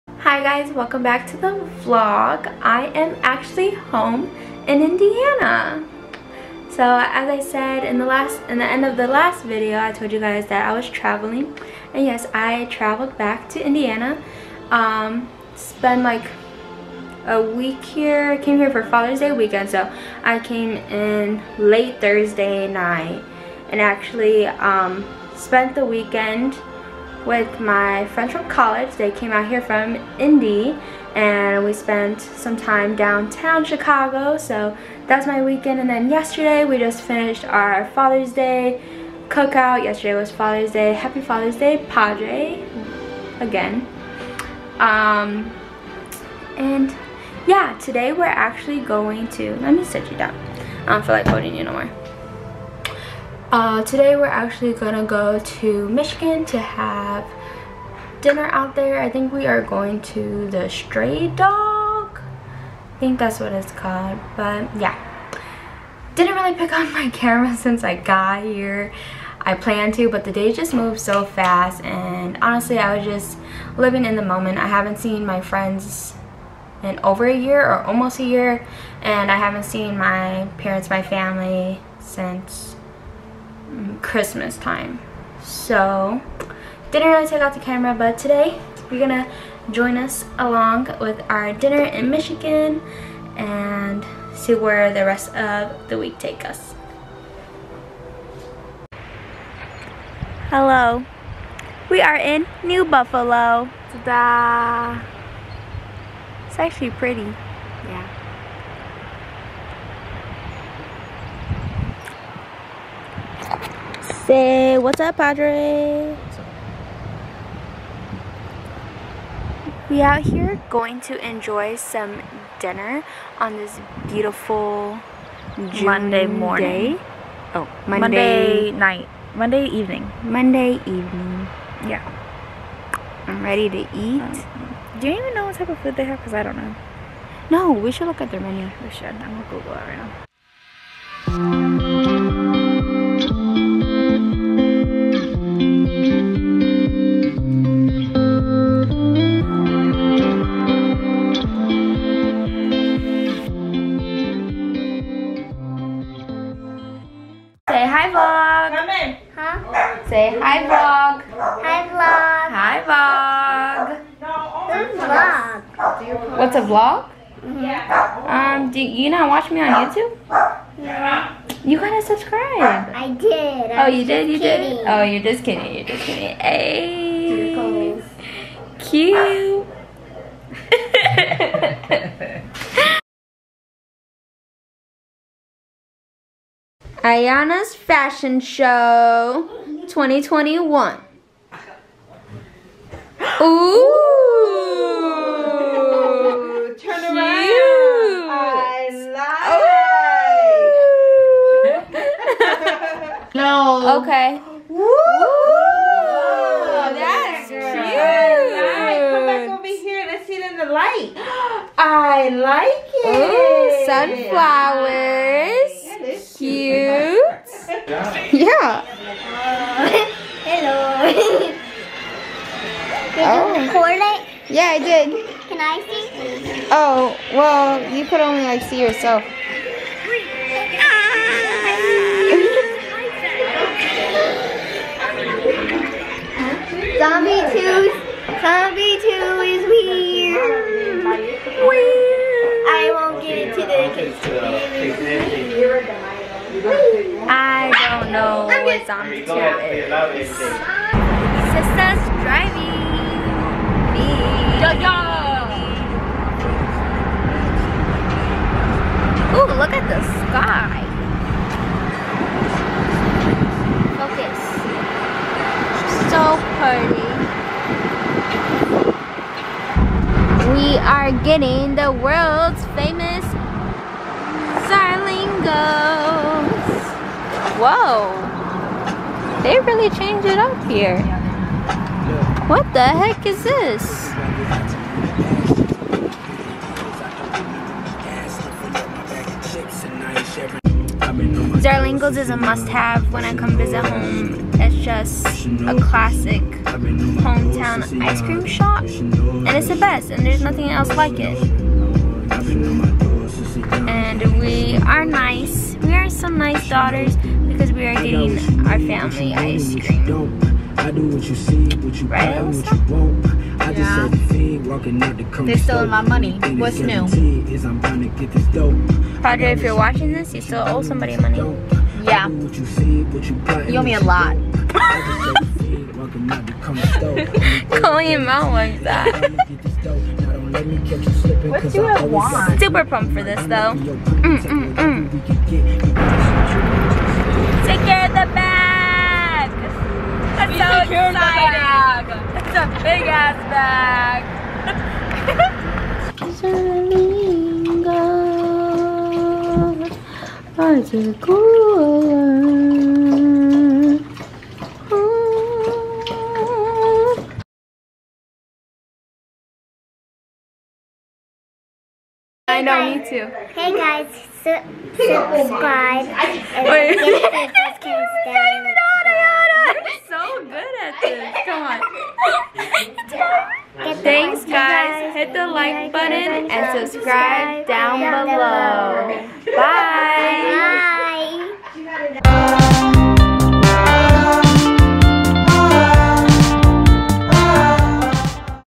VLOG: Family visit!
I haven't been home since Christmas so I thought now would be a great time to make a SAFE trip to visit my family.